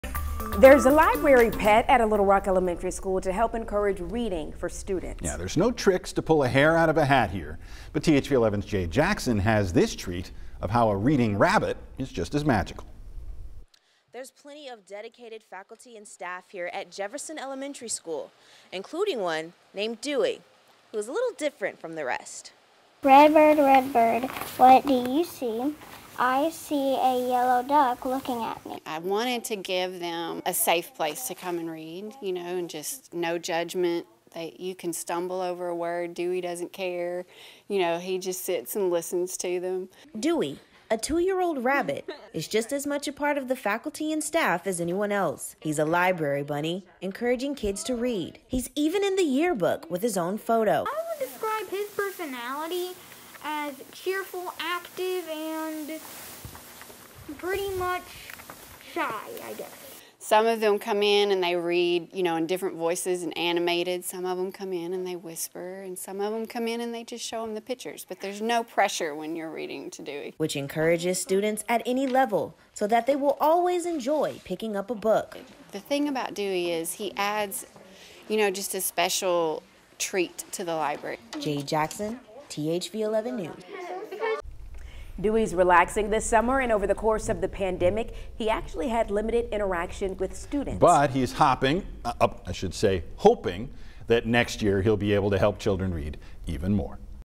This library's pet rabbit helps students to love reading
Some students read to Dewey with different voices that are animated. Some come in and whisper. Others just come in to show him pictures.